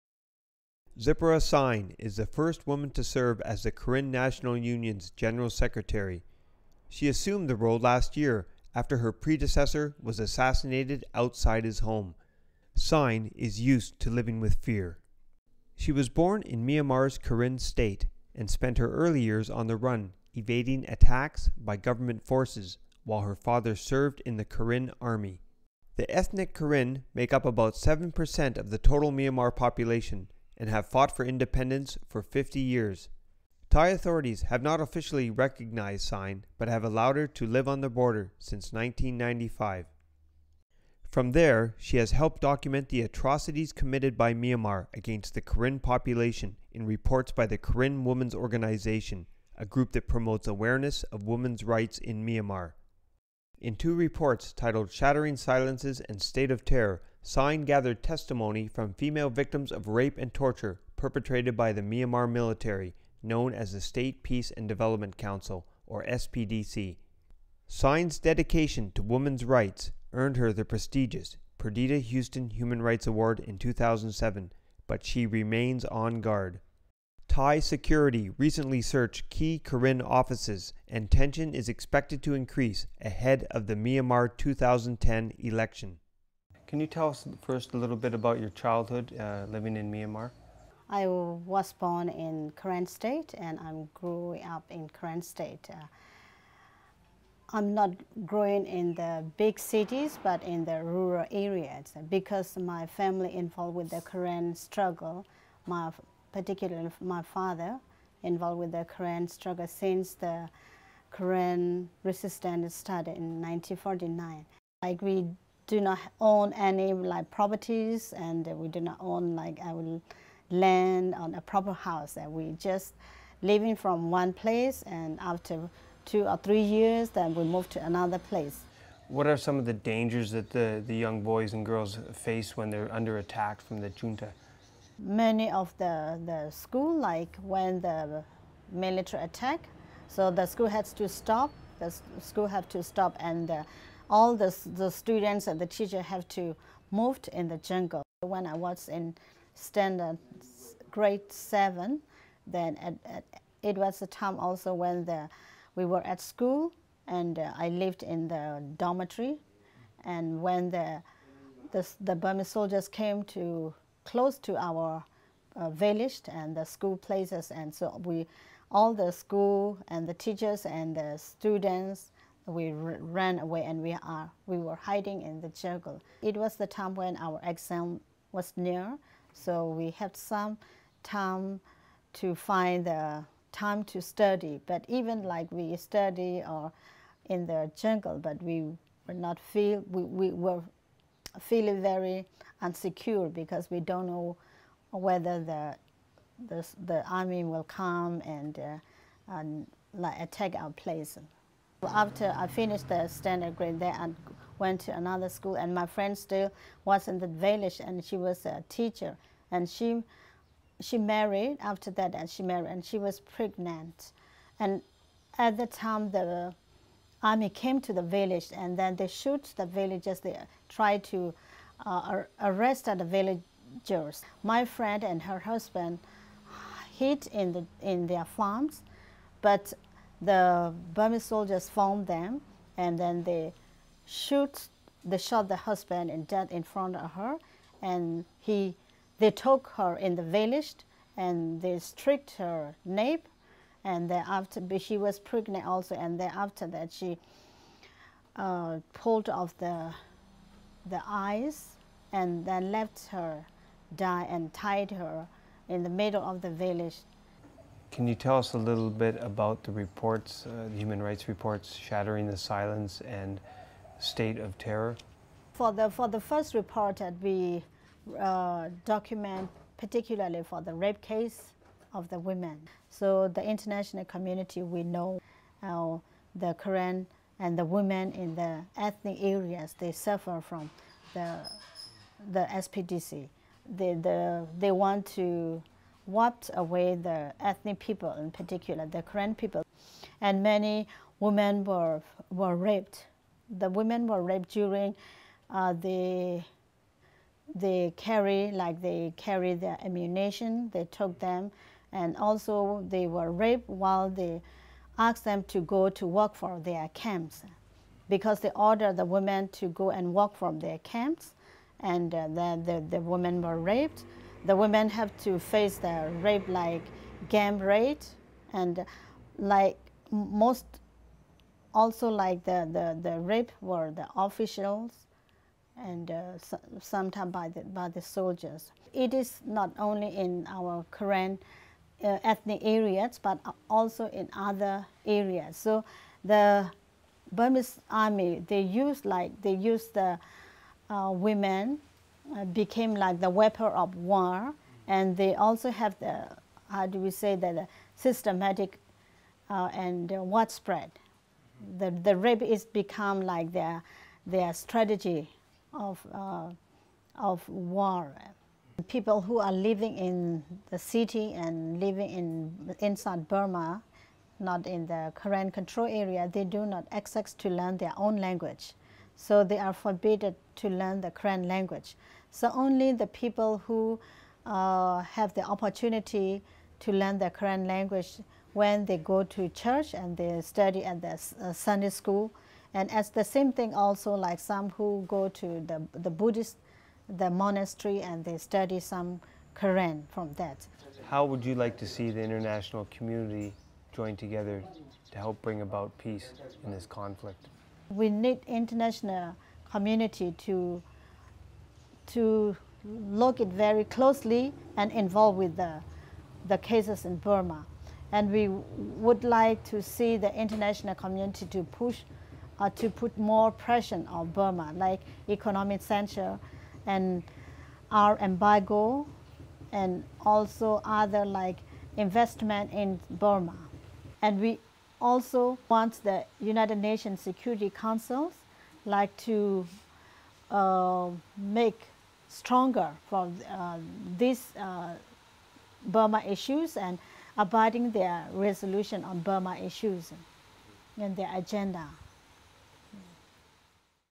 Karen Leader Zipporah Sein On Women's Rights in Myanmar
Zipporah Sein is the first female general secretary of the Karen National Union, which represents the Karen people in Myanmar.